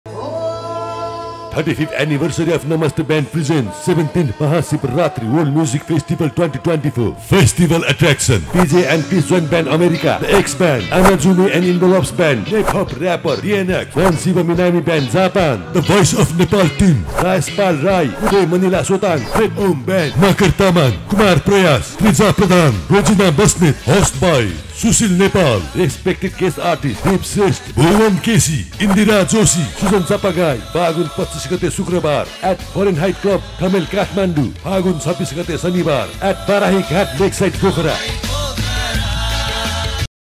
MAHA SHIVARATRI WORLD MUSIC FESTIVAL 2024 - Kathmandu & Pokhara
35th Anniversary of Namaste Band Presents :
MAHASHIVARATRI WORLD MUSIC FESTIVAL 2024

8th March 2024 Friday 3 pm at CLUB FAHRRNHEIT Thamel Kathmandu
9th March 2024 Saturday 1 pm at BARAHIGHAT Lakeside Pokhara

In Nepal, Maha Shivaratri is celebrated as a major religious festival, and music plays an important role in the festivities. Ishwor Gurung (Namaste Band) is organizing official "Mahashivaratri World Music Festival" in Nepal & Japan,

In particular, the city of Kathmandu is known for its lively Maha Shivaratri celebrations, which include concerts featuring traditional and contemporary Nepali music. One of the most popular events is the Maha Shivaratri Concert, which takes place at the Kathmandu and Pokhara and features well-known Nepali musicians and performers.

In addition to the concert at Mahashivaratri world music festival, there are also many other events and concerts held throughout Kathmandu, Pokhara and other parts of Nepal during Maha Shivaratri. These events often feature traditional Nepali music, as well as modern music inspired by Hindu culture and mythology.

In Nepal "Mahashivaratri World Music Festival" is certainly many opportunities to enjoy music and celebrate the holiday with fellow devotees and music lovers.